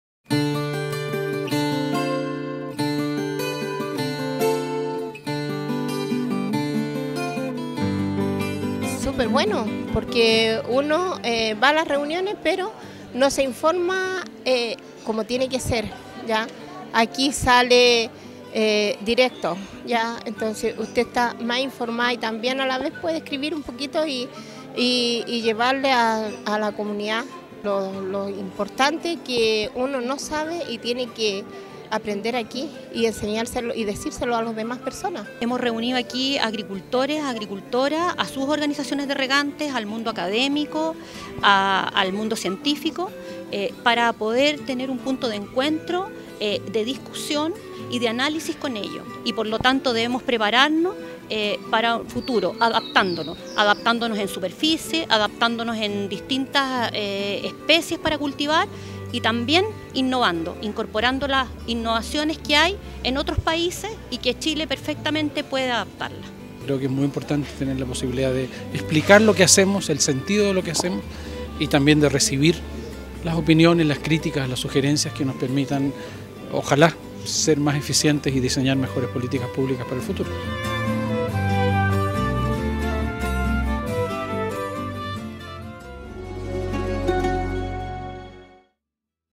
Encuentro Chile Riega 2016
El Ministro de Agricultura, Carlos Furche, inauguró la tercera edición del Encuentro ChileRiega. En la ocasión, la autoridad se refirió a la situación del proyecto de ley enviado al Congreso Nacional que busca generar un nuevo Código de Aguas y de cómo éste va a funcionar para los agricultores.